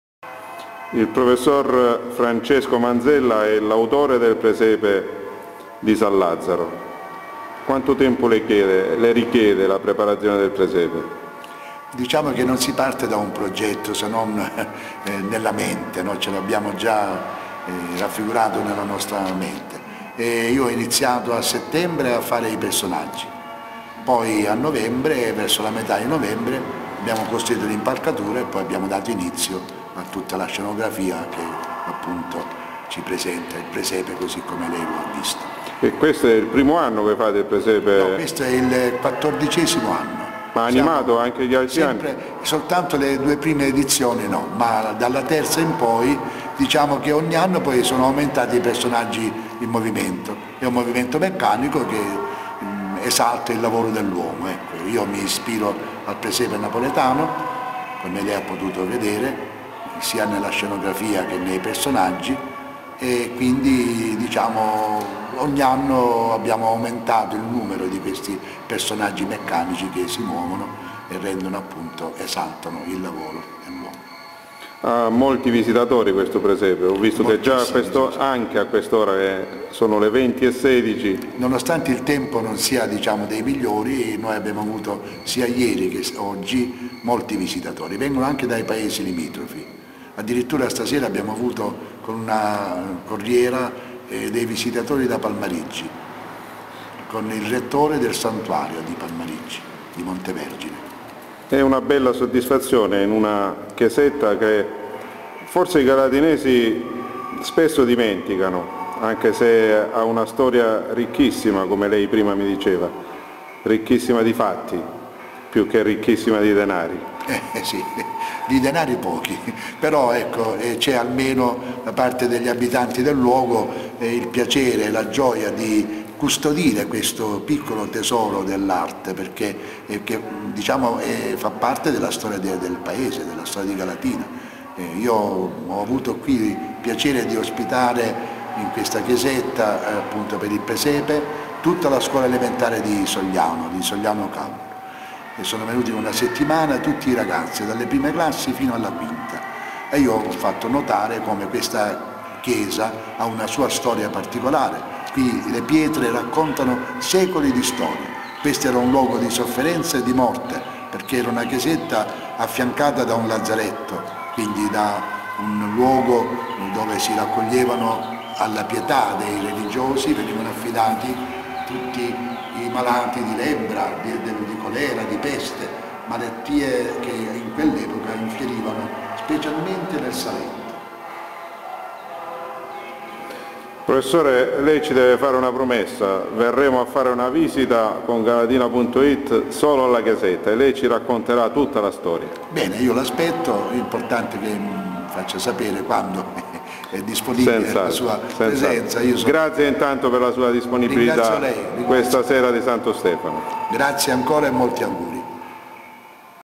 Presepe di San Lazzaro (Francesco Manzella)
26 dic 2007
Francesco Manzella è l'autore del presepio animato della Chiesetta di San Lazzaro (XVI secolo)